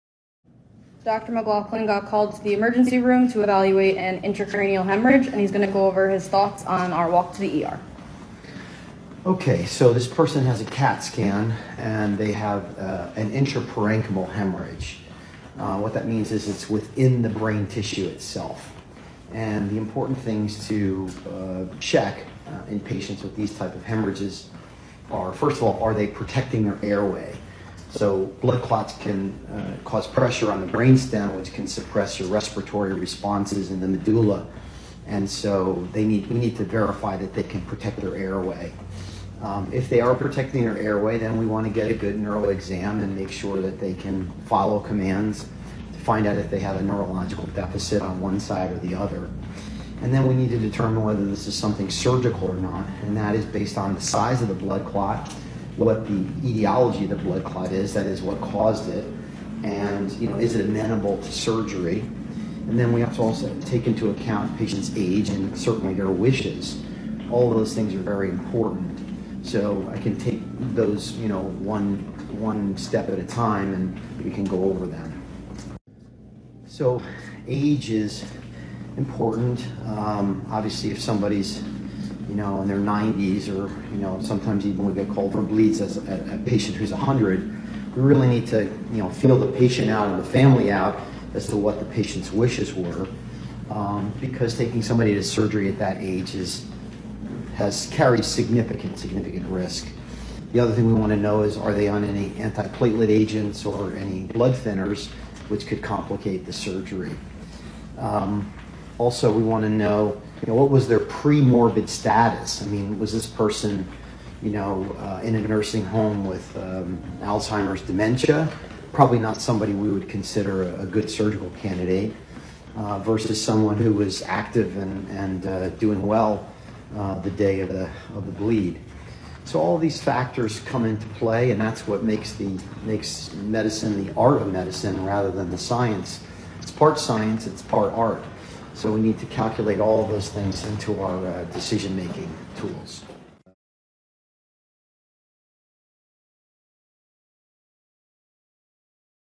Intracranial Hemorrhage: Hallway Walk with Mark McLaughlin MD + Med Student Maggie Fitzpatrick
A patient is in the ER and the CAT scan indicates appears to be an Intraparenchymal Hemorrhage. Dr. McLaughlin is called to evaluate,

In this video, Dr. McLaughlin explains the diagnostic points, and considerations that would affect potential treatments. No patient care was delayed in the slightest , and no healthcare teams were interfered with in producing this video.